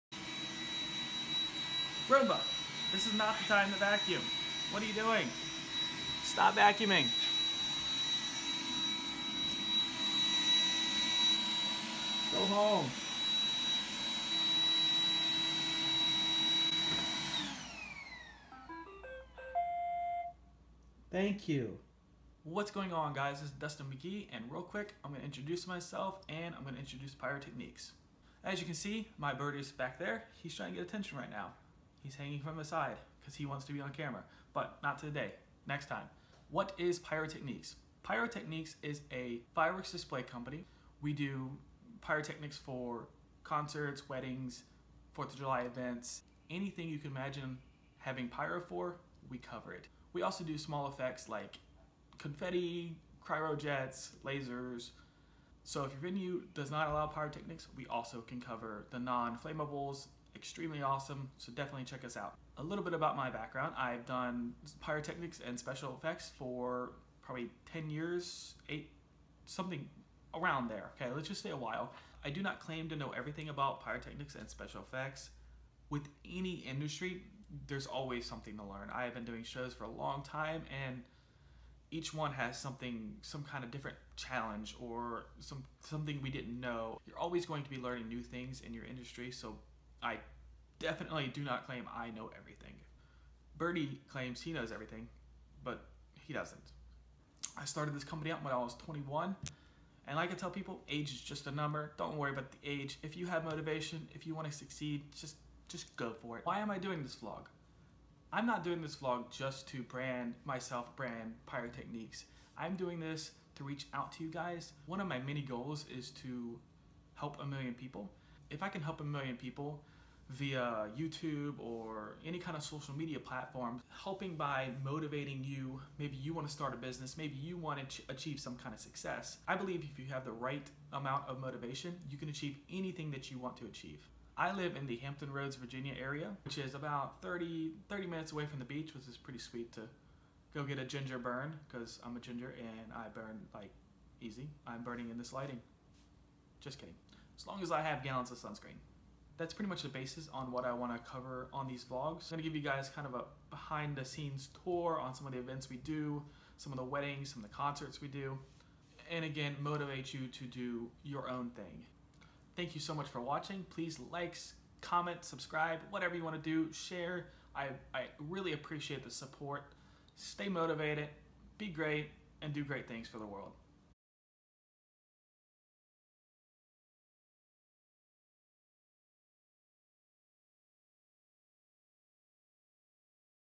Birdie and Roomba try to steal the show!
Here is a quick intro video. I hope this motivates you to follow your dreams. My bird and Roomba decided to try to steal the show!